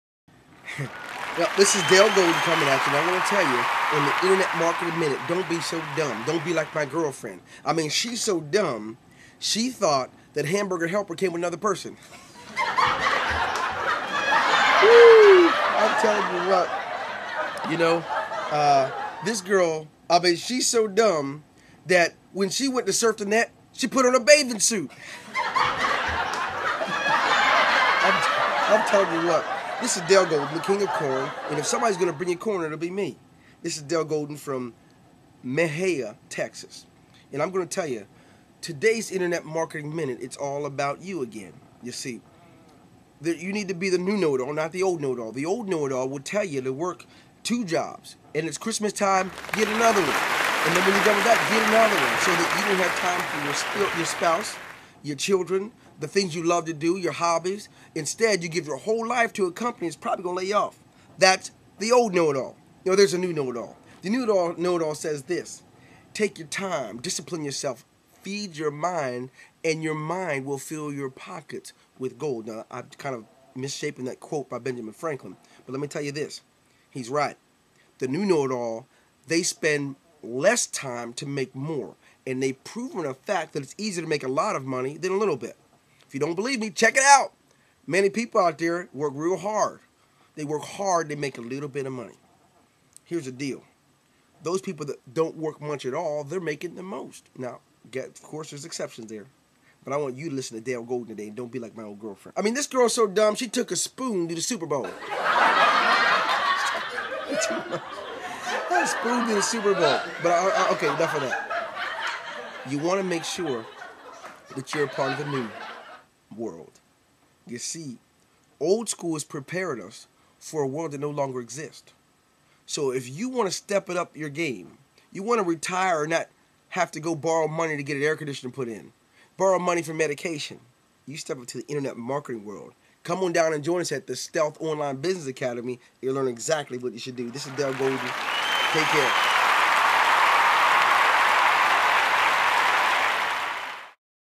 The Internet Marketing Minute9
The Internet Marketing Minute From the studios at the Stealth Online Business Academy Dwayne Golden gives an internet marketing minute by way of VLOG while in character.